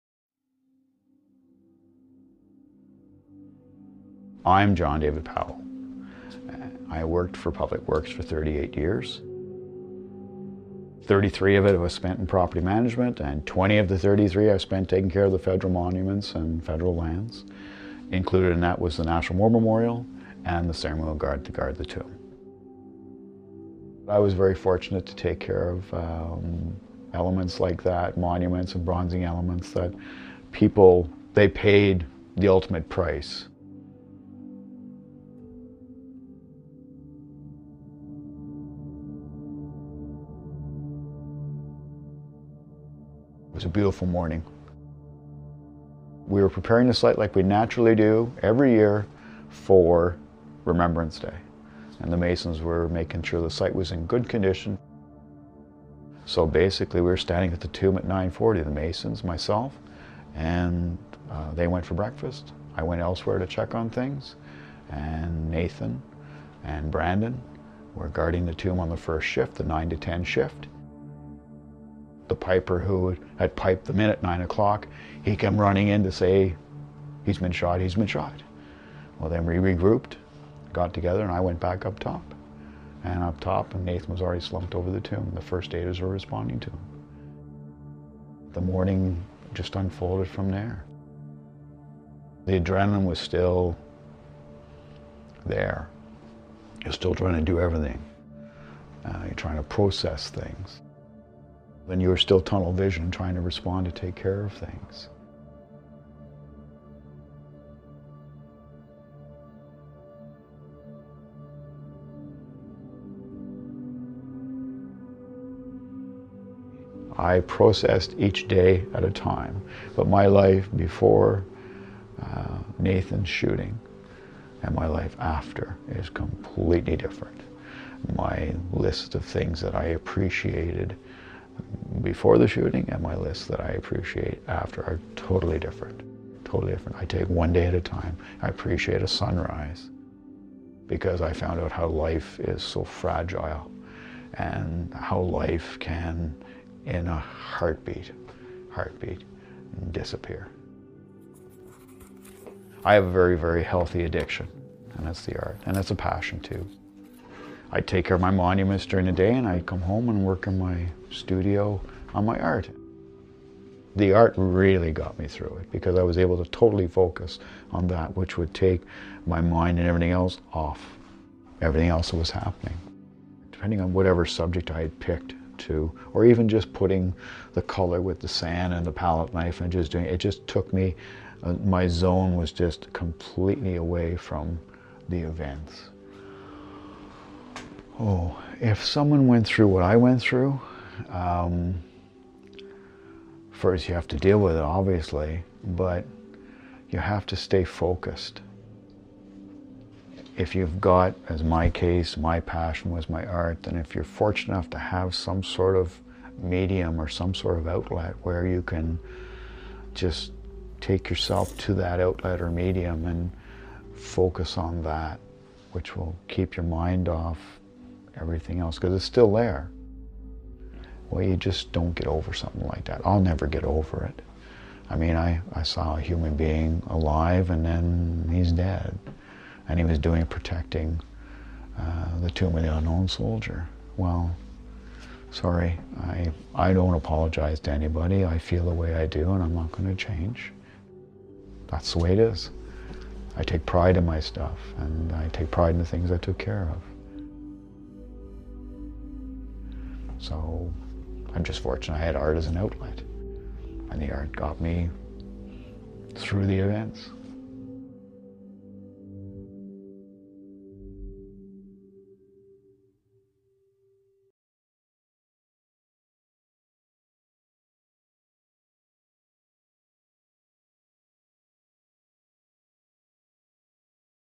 He saw Nathan Cirillo moments after Parliament Hill shooting. Here's how he coped
John-David Powell, a retired property officer who took care of the monuments in Ottawa for 20 years, saw Cpl. Nathan Cirillo moments before and after he was shot on Parliament Hill. He says he's turned to painting to deal with the memories of the October 2014 shooting.